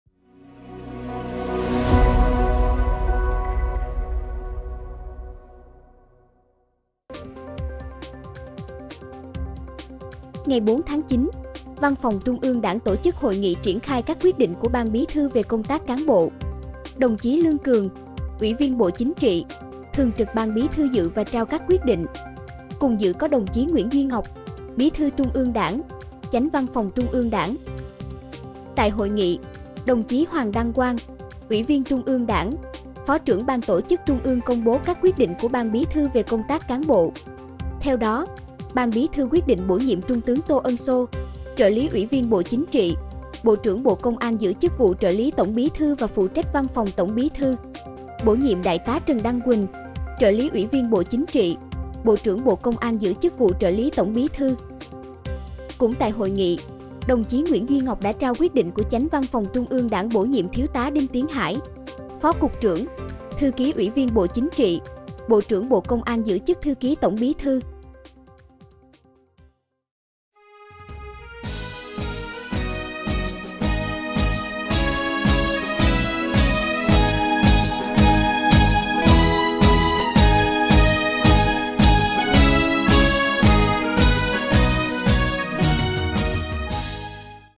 Công bố quyết định bổ nhiệm chức vụ Trợ lý, Thư ký Tổng Bí thư
SGGP | Ngày 4-9, Văn phòng Trung ương Đảng tổ chức hội nghị triển khai các quyết định của Ban Bí thư về công tác cán bộ. Đồng chí Lương Cường, Ủy viên Bộ Chính trị, Thường trực Ban Bí thư dự và trao các quyết định. Cùng dự có đồng chí Nguyễn Duy Ngọc, Bí thư Trung ương Đảng, Chánh Văn phòng Trung ương Đảng.